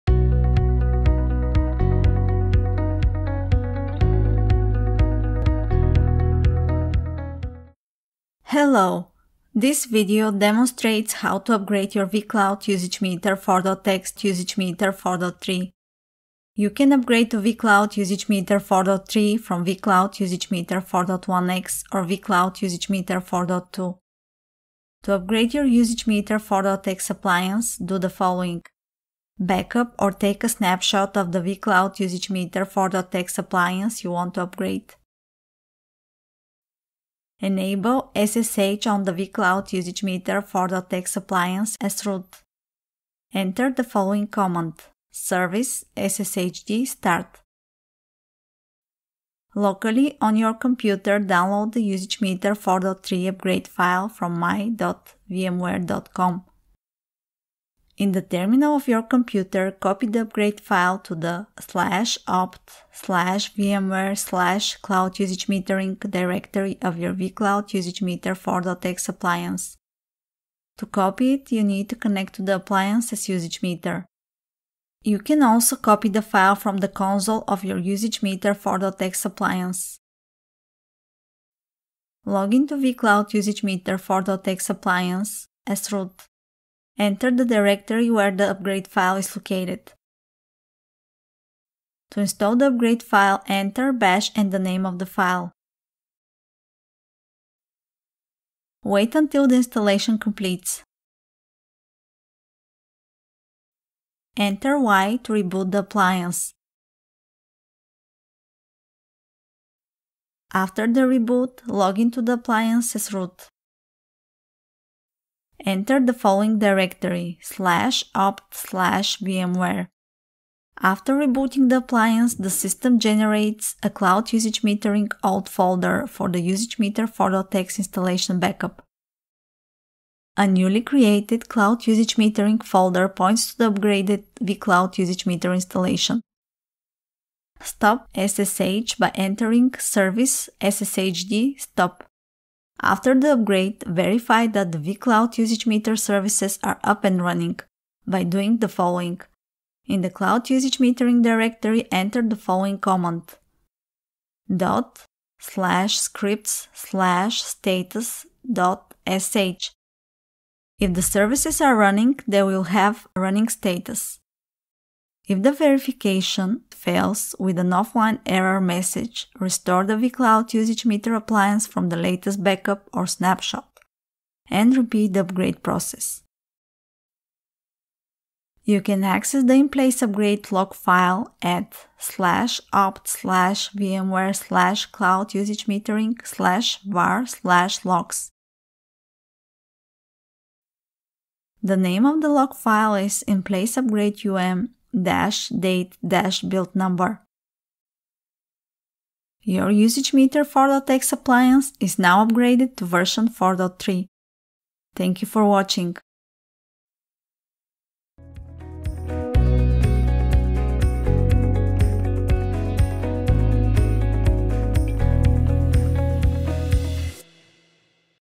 In-place upgrade from vCloud Usage Meter 4.x to vCloud Usage Meter 4.3
From this video, you will learn how to upgrade your vCloud Usage Meter 4.x appliance to vCloud Usage Meter 4.3

Product covered: VMware vCloud Usage Meter